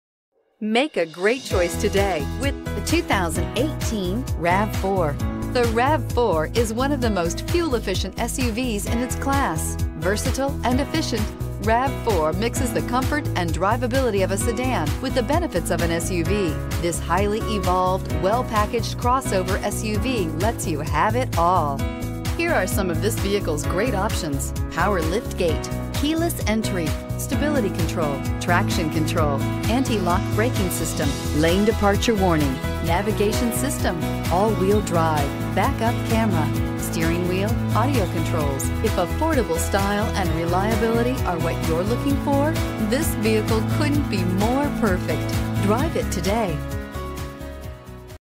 2018 Toyota RAV4 Lee's Summit MO 197754
2018 Toyota RAV4 Limited

Adams Toyota
501 NE Colbern Road

The 2018 Toyota RAV4 deliver more fun with a powerful 176hp 4-cylinder engine for off-the-line thrills. Coupled with the Dual Variable Valve Timing with intelligence technology, the RAV4 is a smart, spirited choice. More room means more space for gear and friends. You can quickly reconfigure the rear seats into a 60/40 split and fold the second-row seats down to create a wide, easily accessible load space with up to 73.4 cubic feet of capacity. RAV4 offers two available audio systems that incorporate navigations, so you can have the perfect soundtrack for all of your adventures. Travel safely with more peace of mind. RAV4 comes standard with a suite of six advanced safety features designed to help keep you out of harms way.